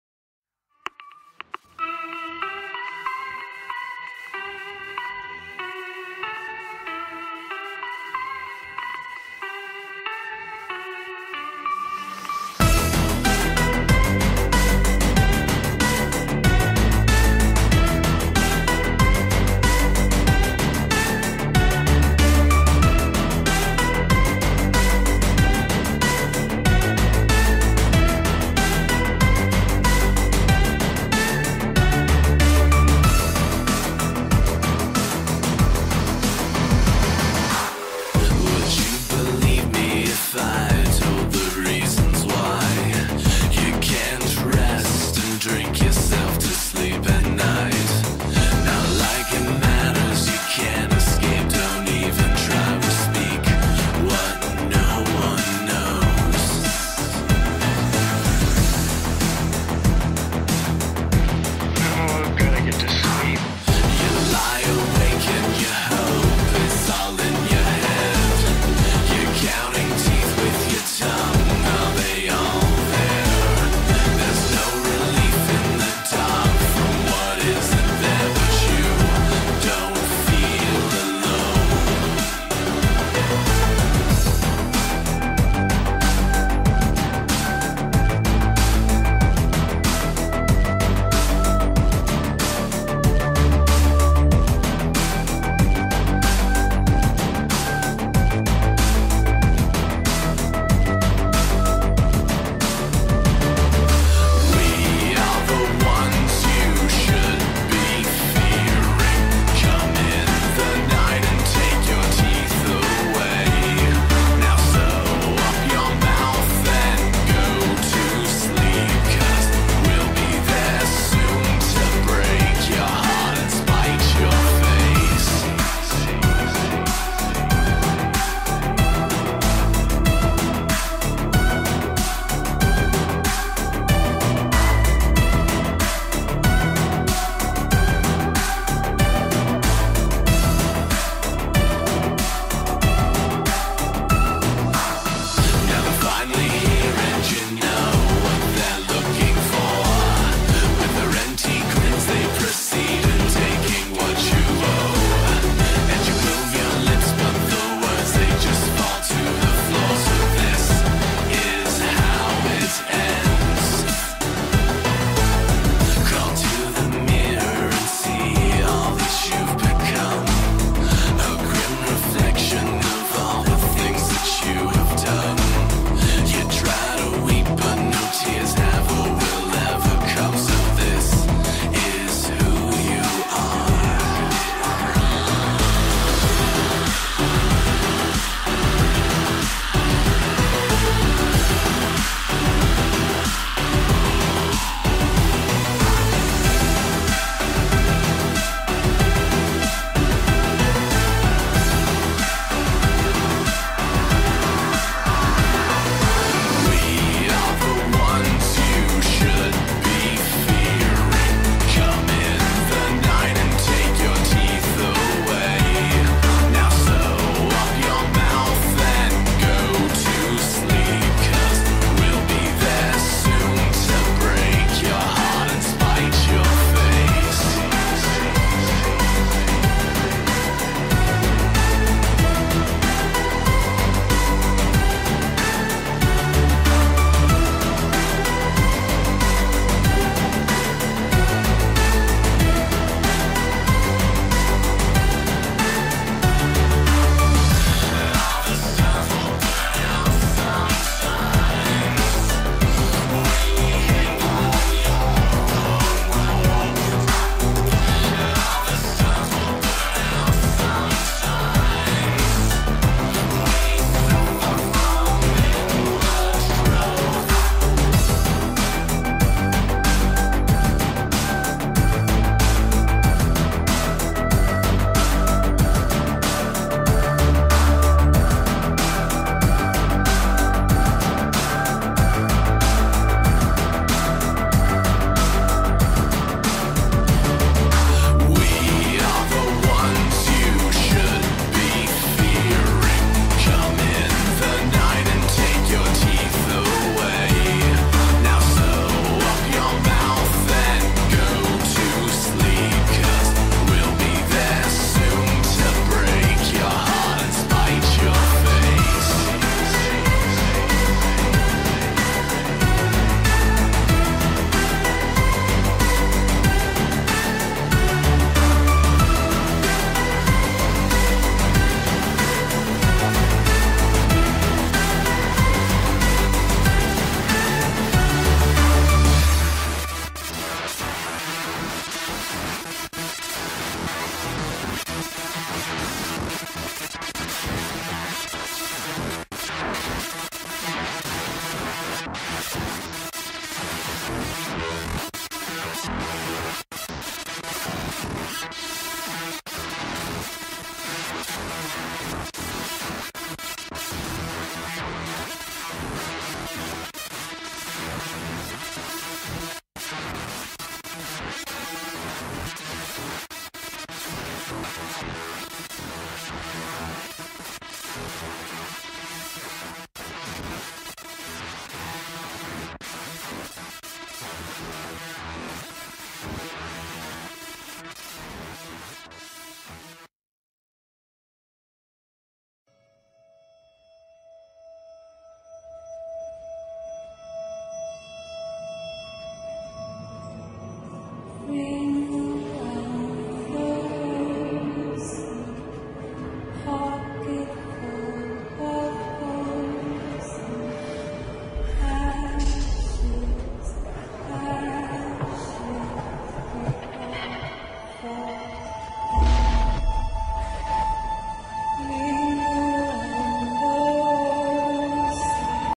LIKE REAL OLD AND SHIT 'n CRINGE: Jeff the killer Speedpaint: Want to see some more?
This time I draw Jeff the killer :D Very bloody and almost naked one but hope you like it ^^ If you liked hit a like and if you want to see more my art subscribe :3